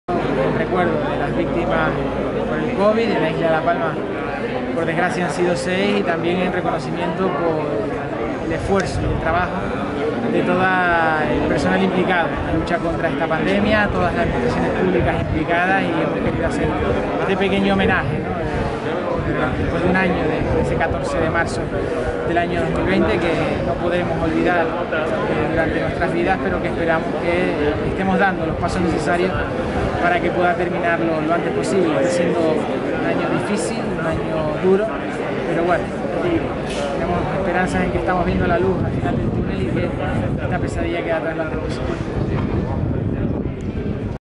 1 año Covid: El Gobierno insular destaca la unidad como clave para afrontar la pandemia en La Palma
Un año de Covid: El Gobierno insular destaca la unidad como clave para afrontar la pandemia en La Palma